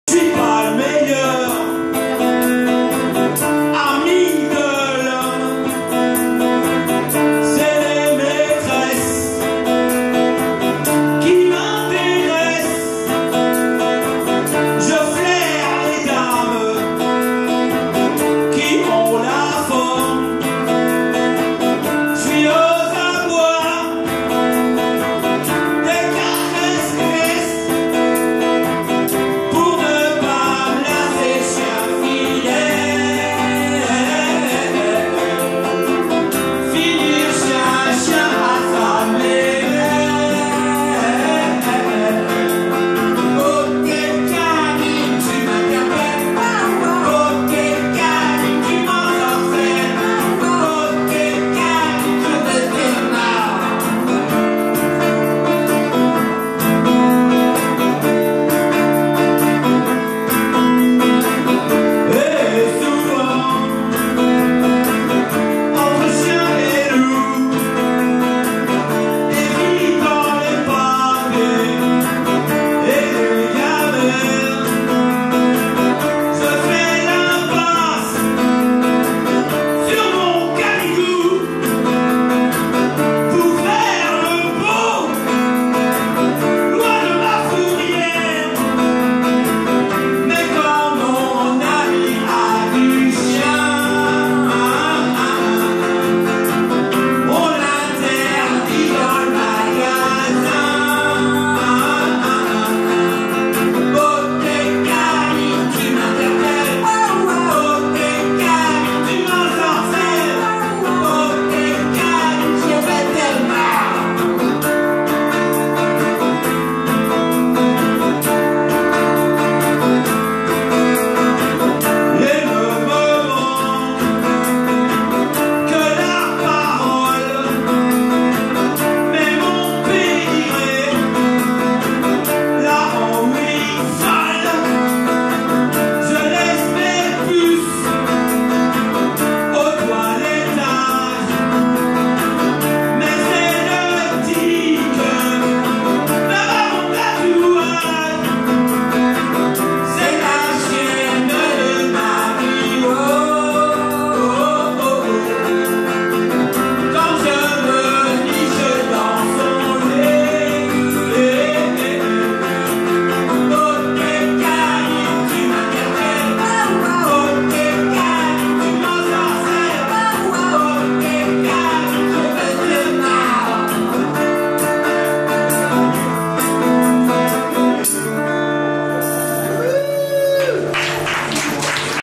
Justesse Social Kleps au Gibus
Extrait du concert de Justesse Social Kleps au Gibus le 13/07/17